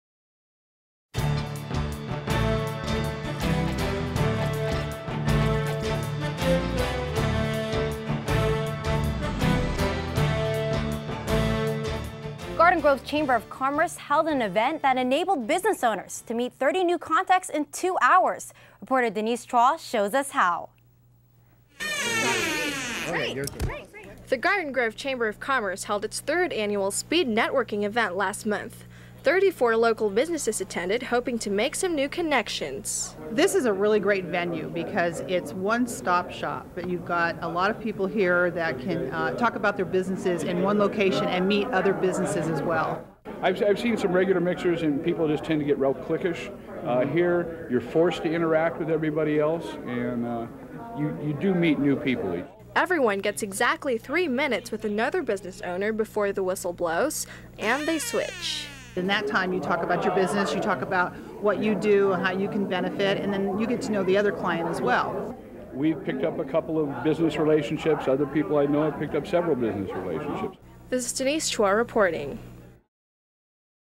Speed Networking
The Garden Grove Chamber of Commerce held an event that enabled business owners to meet 30 new contacts in two hours.  Garden Grove TV3 reporter Denise Chuah shows us how.